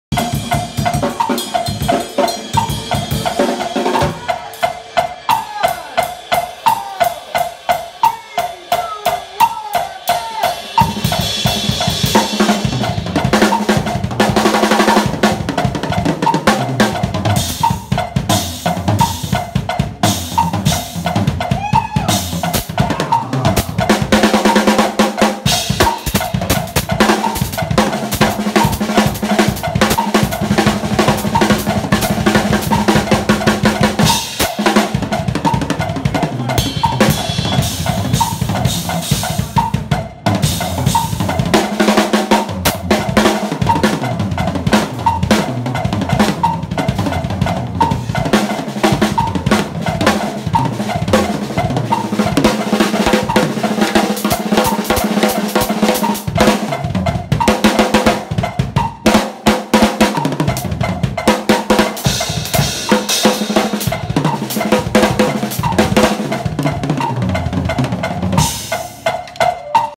Drum Exercise With Metronome- Nathan Caraballo
just an exercise my homies came up with chopping straight through non stop with a metronome see if u can keep coming up with ideas